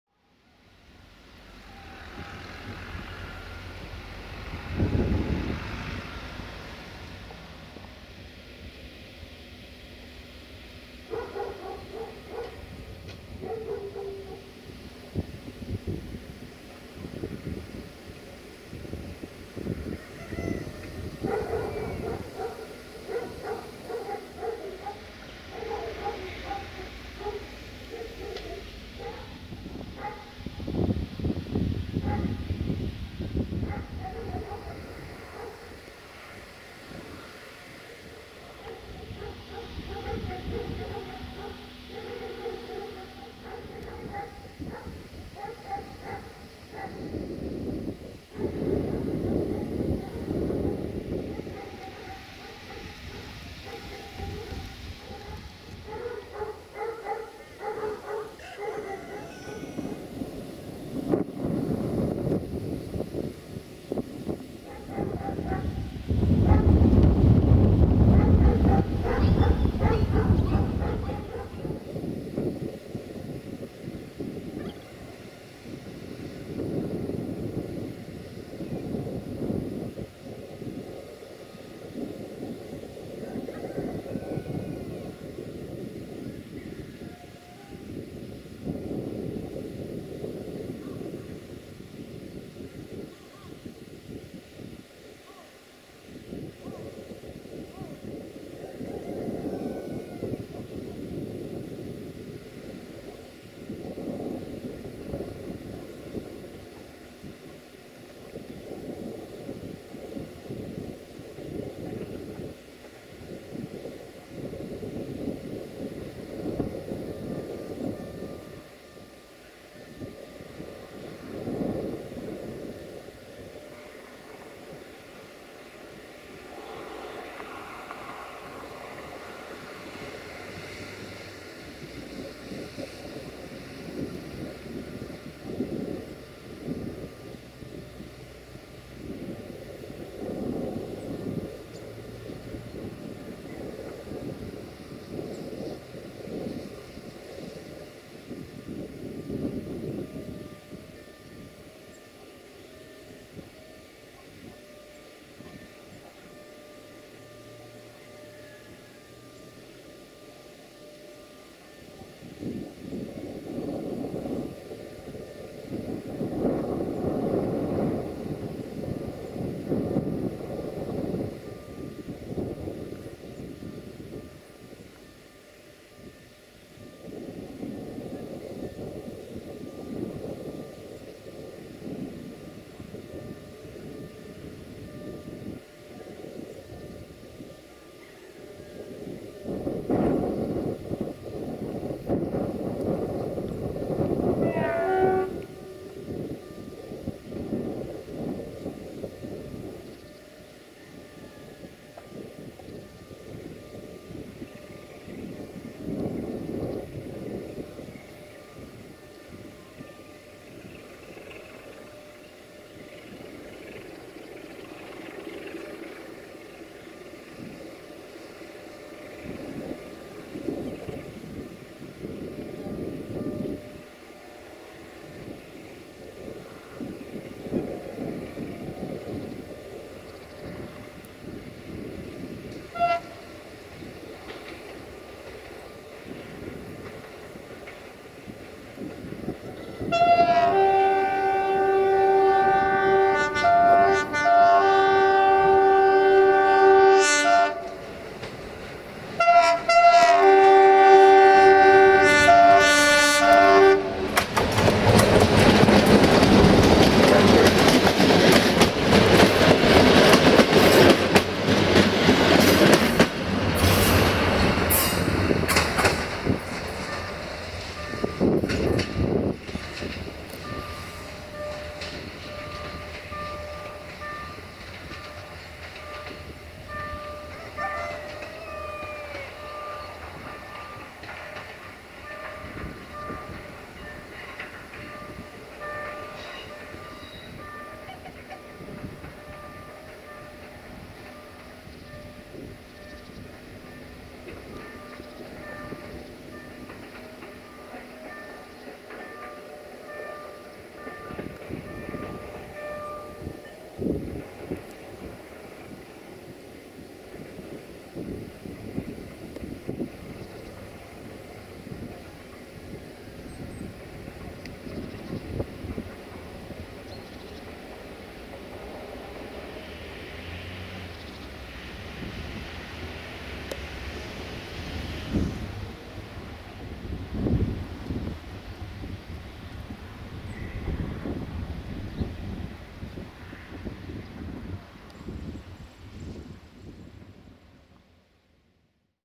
060-DA 60-1228-5 & Mecanic Super Engineer in Gara Săcuieni Bihor Station - 29 January 2020
060-DA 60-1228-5 & Mecanic Super Engineer in Gara Săcuieni Bihor Station - 29 January 2020

1:R4342 - Valea lui Mihai - Săcuineni Bihor - Diosig - Biharia - Episcopia Bihor - Oradea 060-DA/LDE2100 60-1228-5 RO-SNTFC

29 January 2020